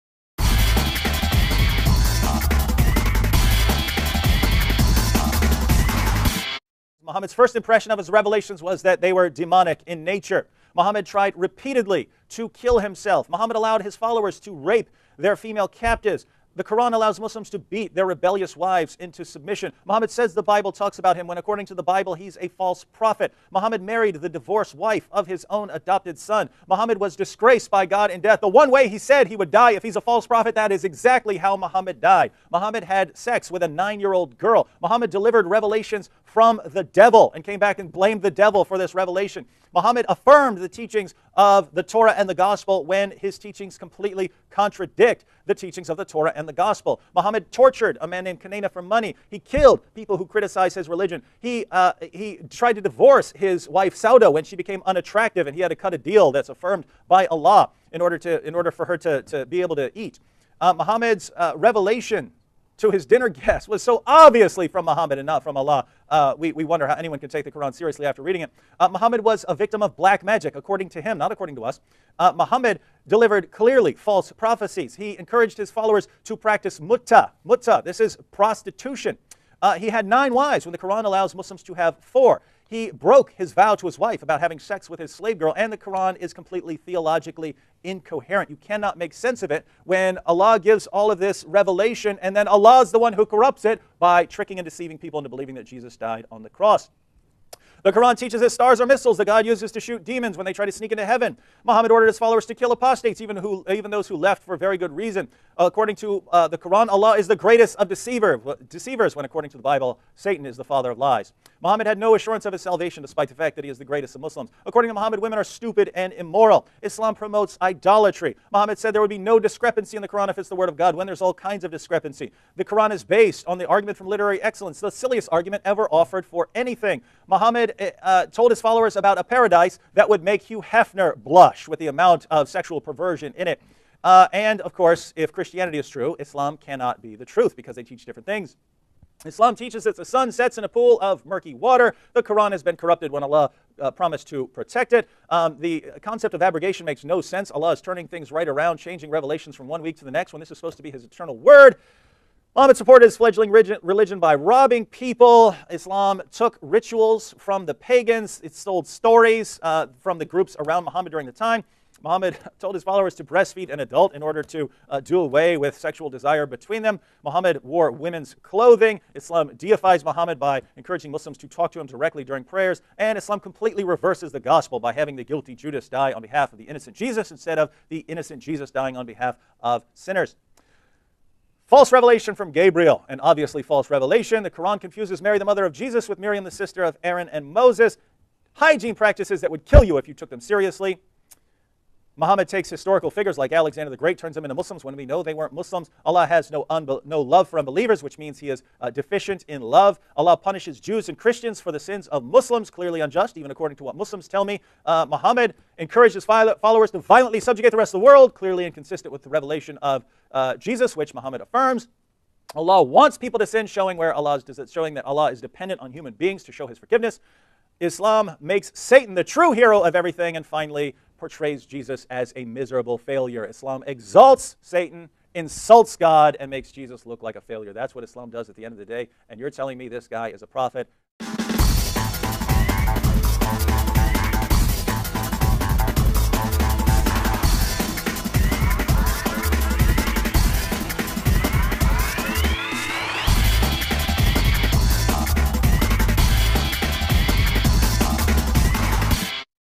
Reasons why Muhammad was not a prophet | David wood
solid believer,
50 Reasons Muhammad Was Not a Prophet (in Under Five Minutes)